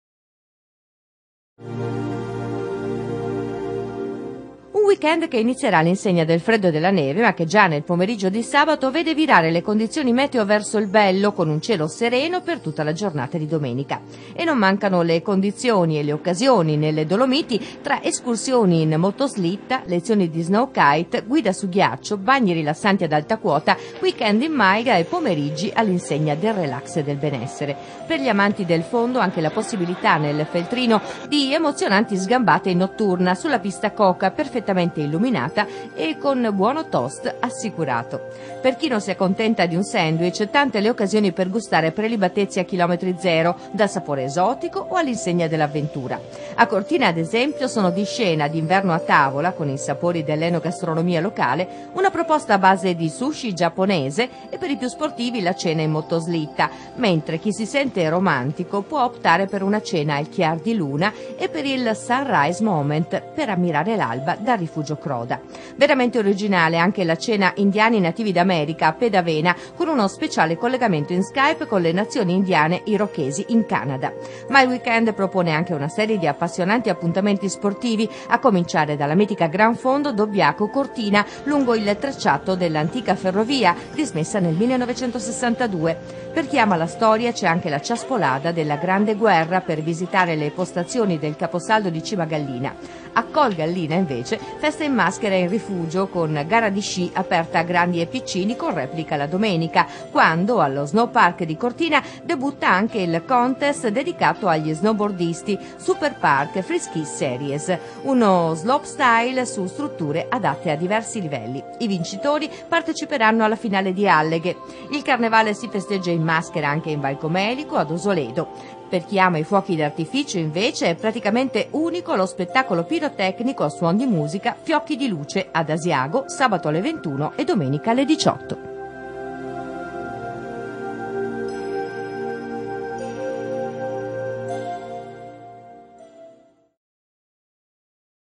SPORT E DIVERTIMENTO
Week end  di sport e divertimento in montagna. Anche se sabato è previsto il passaggio di una perturbazione, la domenica sulle nevi sarà all'insegna del sole.